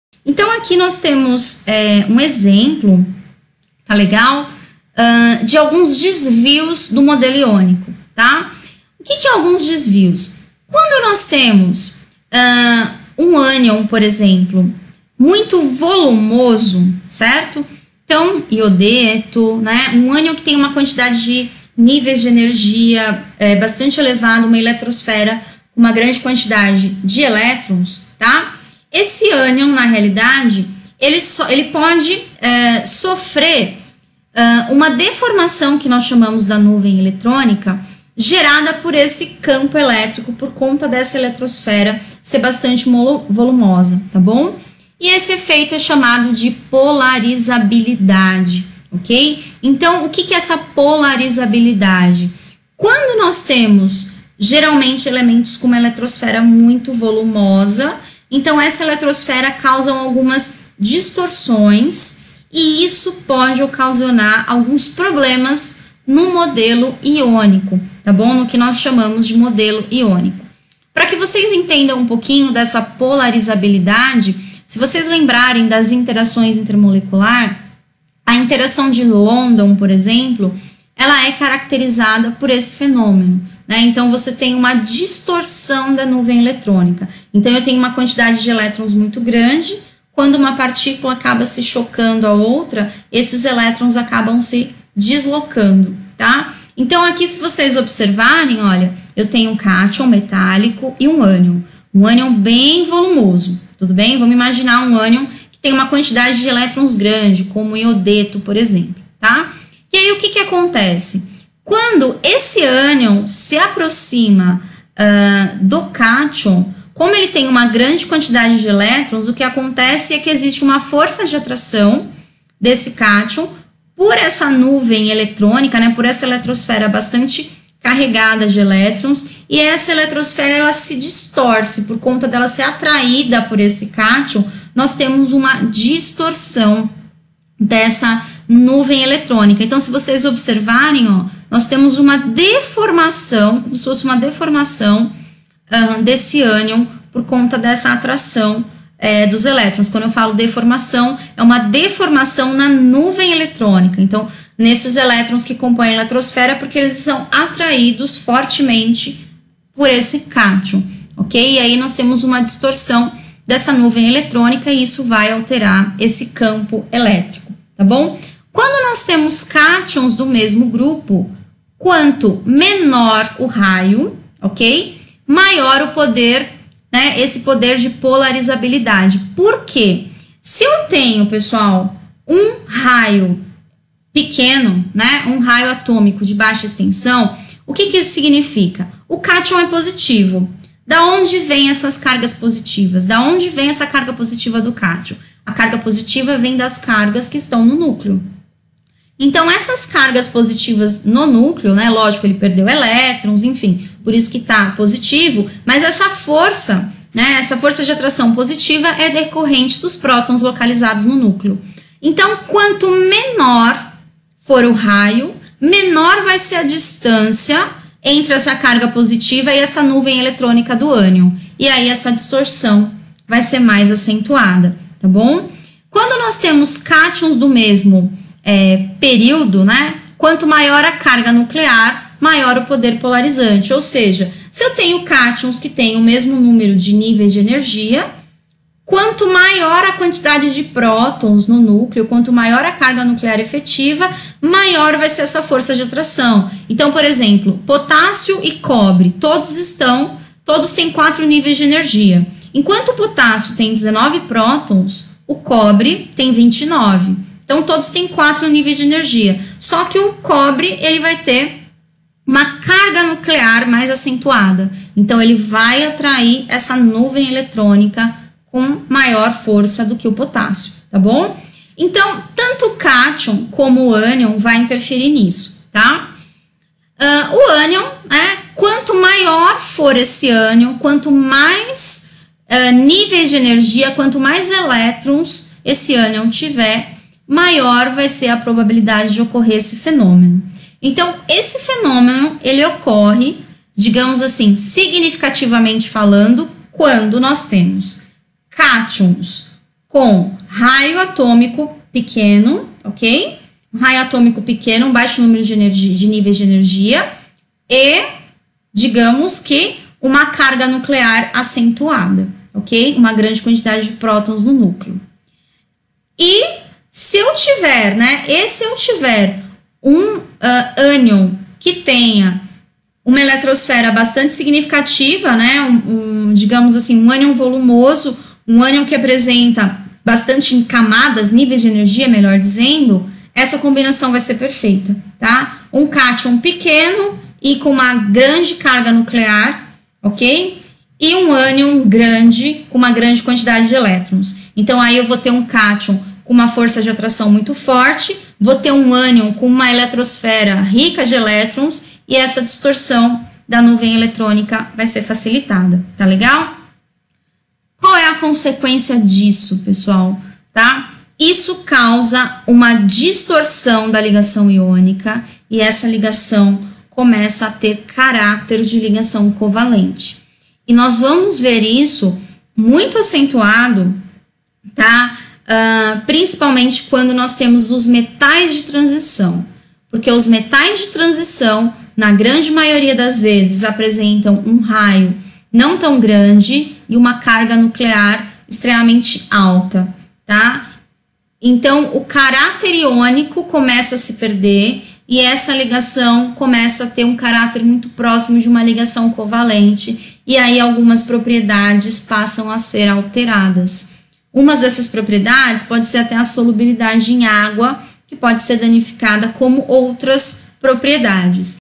Ligações iônicas com caráter covalente - O efeito da polarizabilidade
No vídeo de hoje, vamos explorar o fascinante efeito da polarizabilidade no modelo iônico. Vamos discutir como a polarizabilidade dos íons afeta a força das interações eletrostáticas em um composto iônico. Vamos analisar como os íons podem deformar a nuvem eletrônica do outro íon próximo, levando a mudanças nas distâncias e nas forças entre eles. Além disso, vamos abordar como a polarizabilidade influencia as propriedades físicas e químicas dos compostos iônicos, como seus pontos de fusão e ebulição. Prepare-se para desvendar os segredos do efeito polarizabilidade no modelo iônico e entender como essa característica dos íons é fundamental para compreender o comportamento dos materiais sólidos.